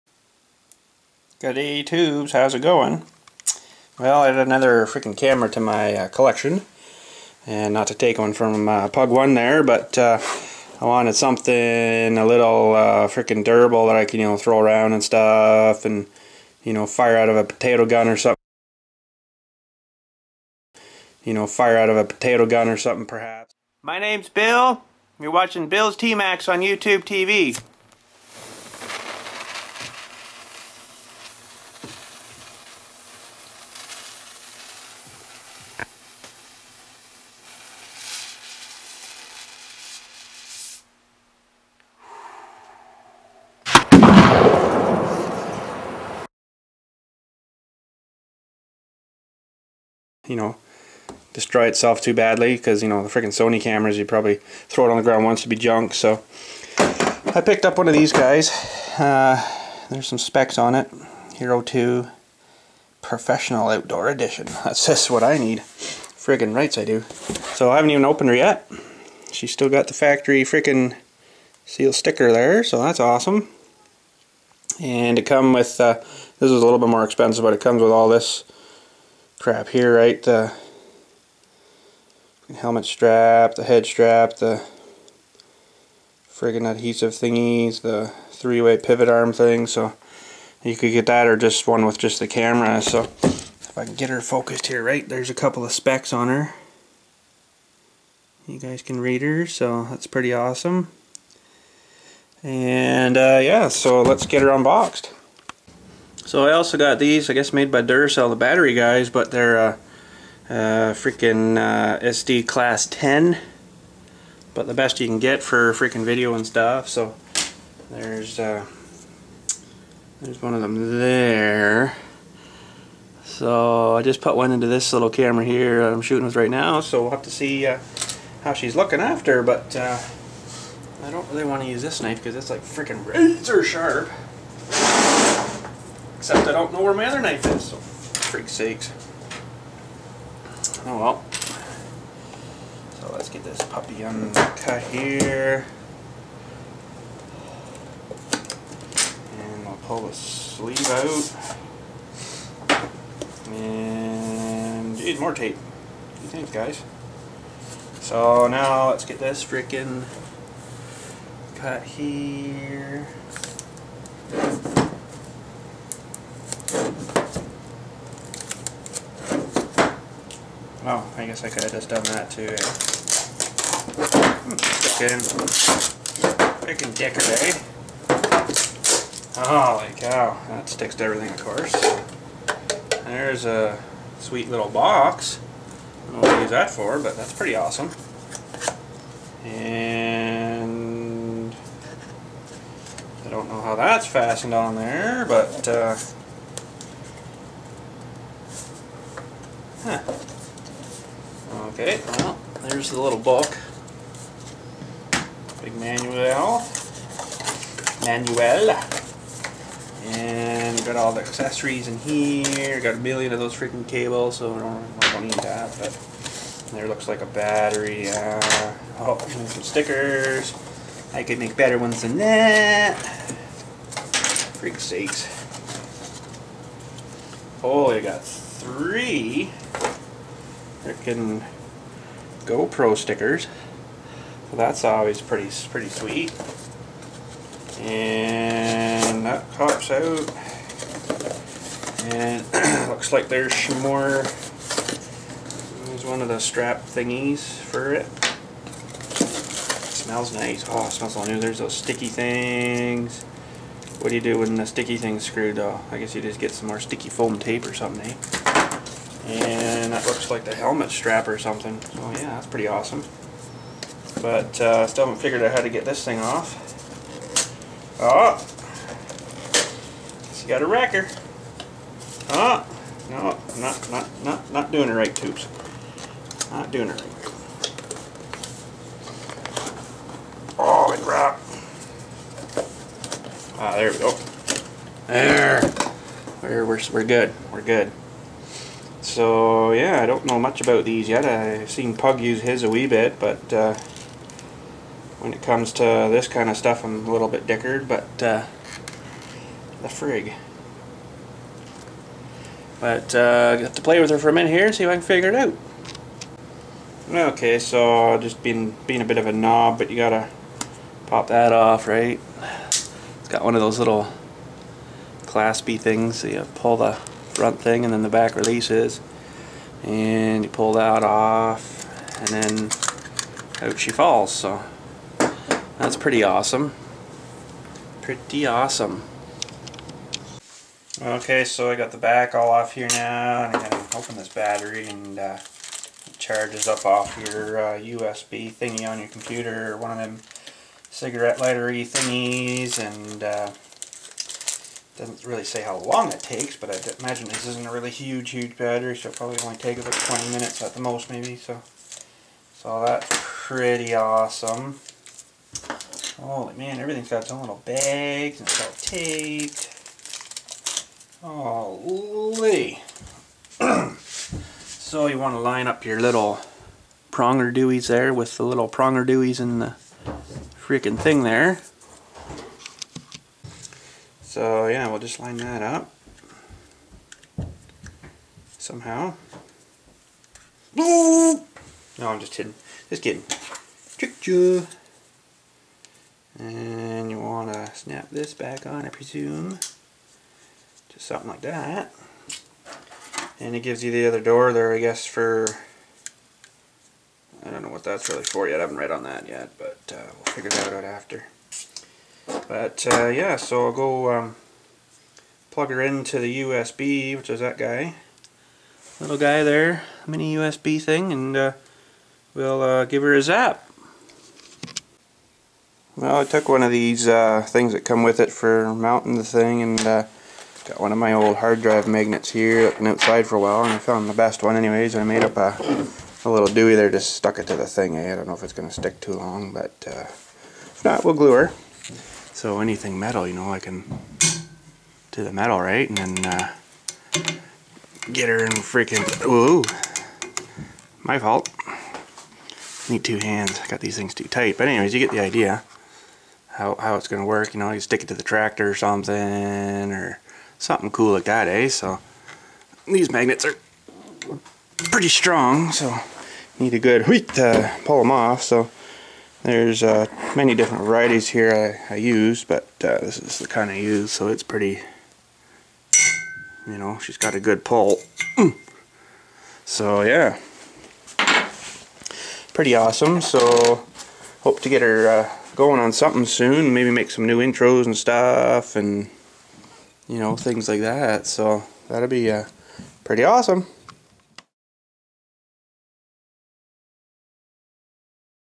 new gopro to mess around with
i wanted something to get right into the action and not have to worry about it to much  so i got a go pro